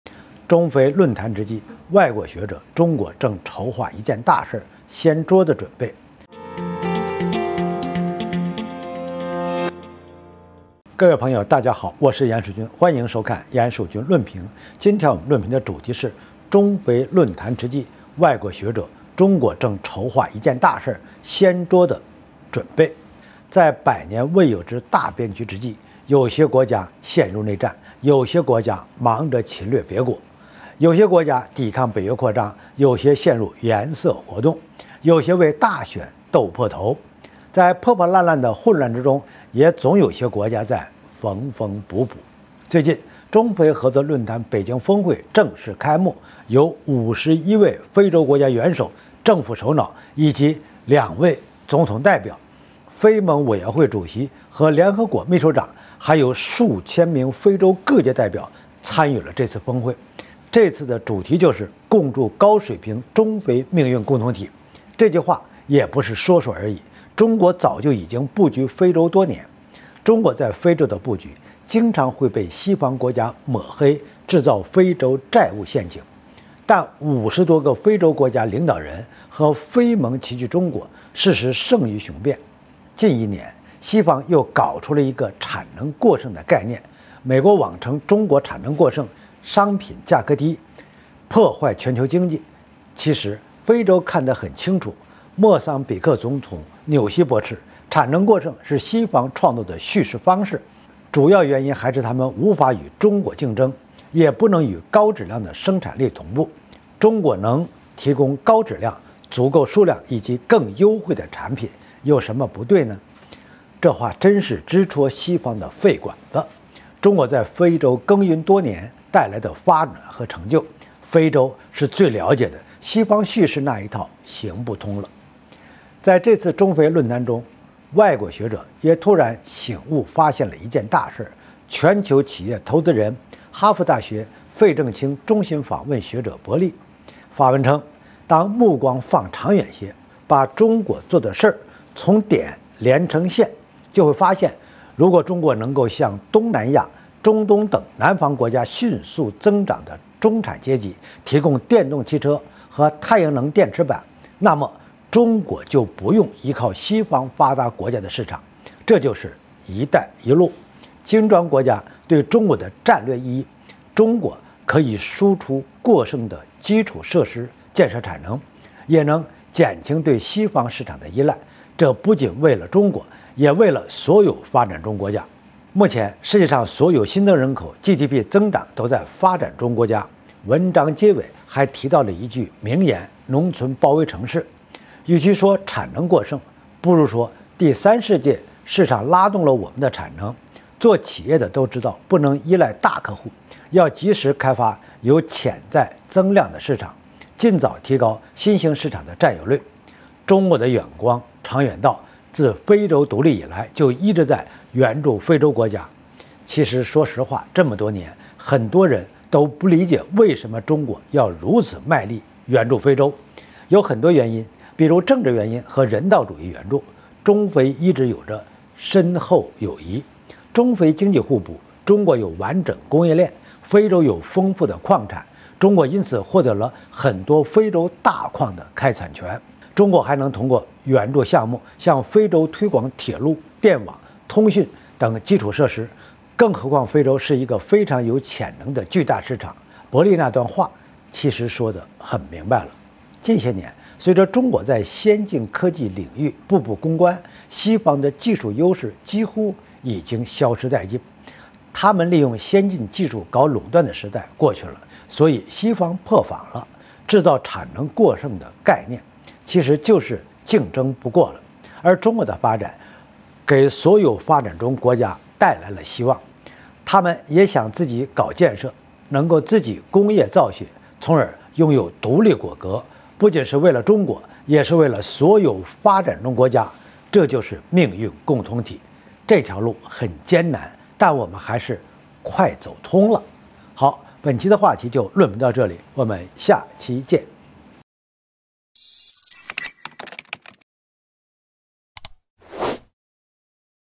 中非論壇之際，外國學者：中國正籌劃一件大事，西方破防
中非論壇之際，外國學者：中國正籌劃一件大事，掀桌的準備。